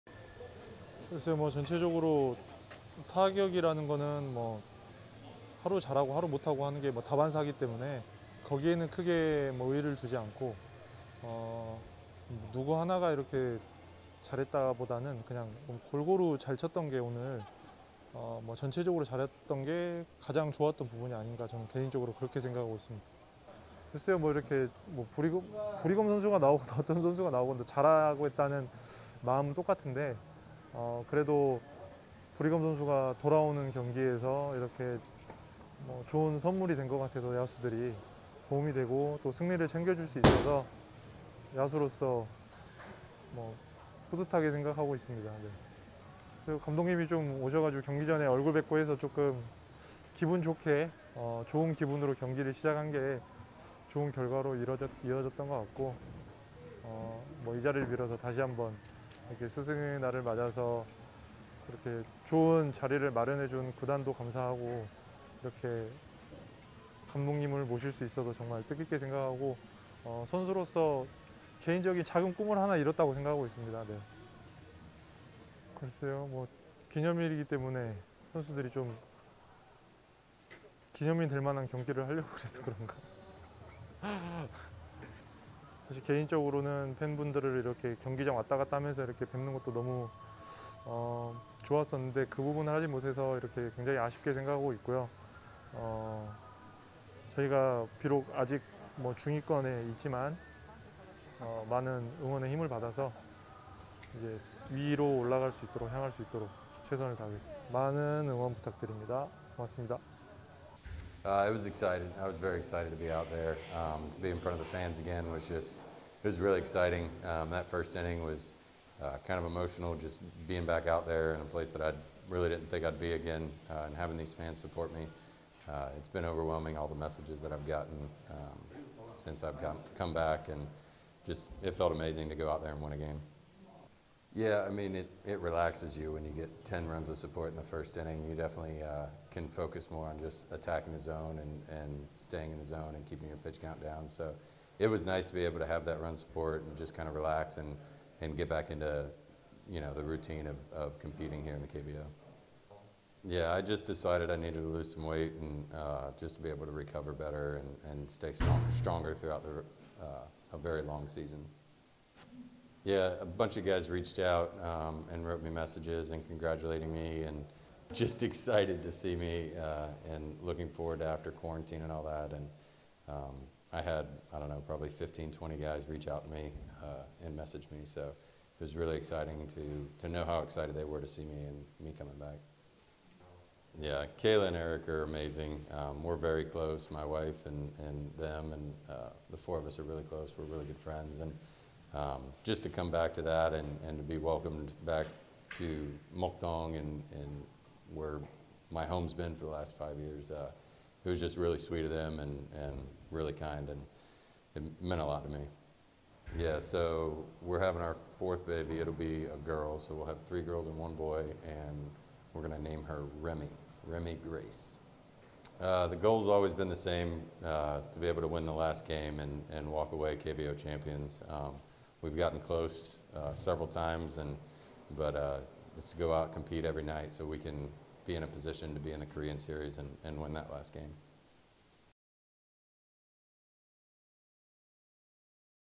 4타수 3안타 1타점 서건창, 5 ⅔이닝 7K 무실점으로 복귀한 브리검 | 수훈선수 인터뷰 (5.15)
매일 기념일이라면😂